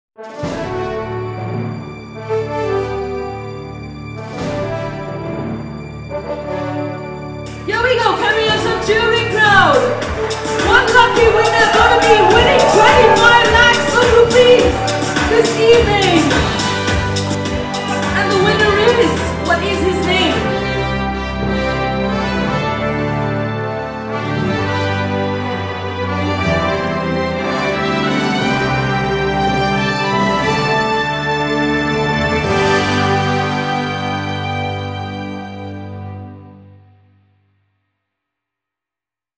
Winner of the November Golden Ticket at Deltin Royale || Deltin Life
Here's the winner of the Golden Ticket jackpot at Deltin Royale held in November, 2015.